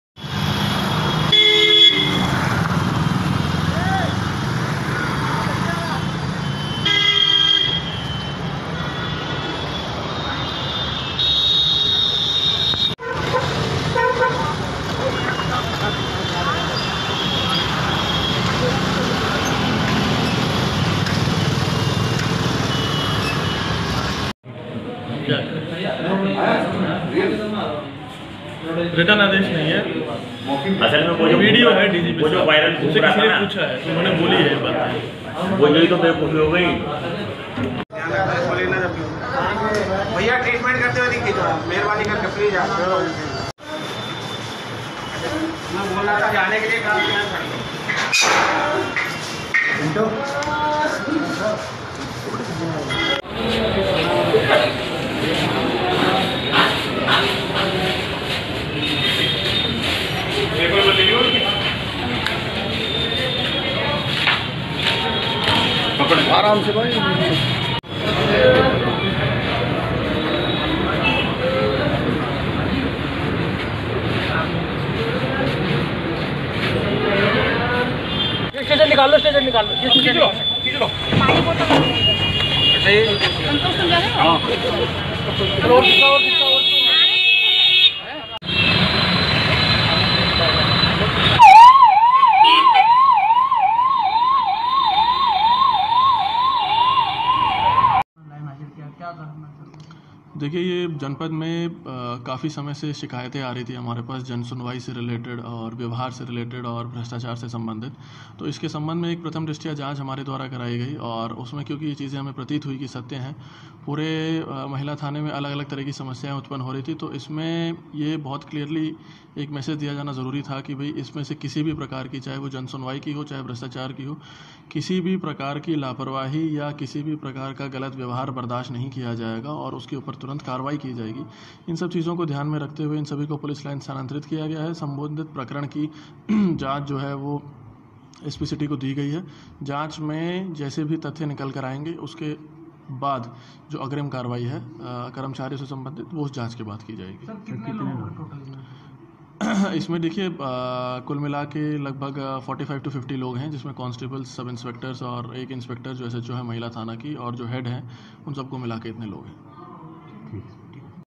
मुज़फ्फरनगर महिला थाने पर गिरी गाज.. एसएसपी ने पूरे महिला थाने को किया लाइन हाजिर
मुज़फ्फरनगर ।  मुज़फ्फरनगर एसएसपी अभिषेक यादव ने बड़ी कार्यवाही करते हुए पूरे महिला थाने को लाइन हाजिर कर दिया है। एसएसपी अभषेक यादव को महिला थाने में लगातार भ्रष्टाचार और अनियमिताओं की शिकायतें मिल रही थी। जिसको लेकर एसएसपी ने महिला थाने को जाँच के बाद दोषी पाया , और बड़ी कार्यवाही करते हुए महिला थाना प्रभारी सहित महिला थाने में तैनात 50 पुलिसकर्मियों को लाइन हाजिर कर दिया है।

दरअसल मुज़फ्फरनगर एसएसपी अभषेक यादव को काफी दिनों से महिला थाने में लगातार भ्रष्टाचार और अनियमिताओं की शिकायतें मिल रही थी। जिसको लेकर एसएसपी ने एक जाँच टीम गठित कर महिला थाने की जाँच कराई। जाँच टीम द्वारा की गई जाँच में महिला थाने को दोषी पाया गया। जिसके बाद एसएसपी अभिषेक यादव ने बड़ी कार्यवाही करते हुए महिला थाना सहित सहित थाने में तैनात 50 पुलिसकर्मियो को लाइन हाजिर कर दिया है।

पूरे मामले की जाँच एसपी सिटी सतपाल अंतिल को दी गई है। जाँच के बाद अग्रिम कार्यवाही की जाएगी। एसएसपी अभिषेक यादव ने जानकारी देते हुए बताया कि जनपद में काफी समय से शिकायते आ रही थी हमारे पास जन व्यवहार से रिलेटिड और भ्र्ष्टाचार से रिलेटिड। इस संबन्ध में प्रथम दृष्टिया जांच हमारे द्वारा कराई गई और क्योकि उसमे कुछ चीजे हमे प्रतीत हुई की सत्य है। पूरे महिला थाने में अलग-अलग प्रकार की समस्याएं उत्पन्न हो रही थी। इसमें एक मेसिज दिया जाना बहुत जरुरी था की इसमें से किसी भी प्रकार की समस्या चाहे वो जन सुनवाई की हो या भ्र्ष्टाचार की हो। किसी भी प्रकार की लापरवाही या गलत जन व्यवहार बर्दास्त नहीं किया जायेगा और तुरंत कार्यवाही की जाएगी। सभी को ध्यान में रखते हुए इस प्रकरण की जाँच एसपी सिटी को दी गई है। जाँच में जैसे भी तथ्य निकलकर आएंगे उसके बाद जो अग्रिम कार्यवाही है कर्मचारियों से संबन्धित वो उस जाँच के बाद की जाएगी। इसमें कुल मिलाकर 45 से 50 लोग है सबको मिलाकर।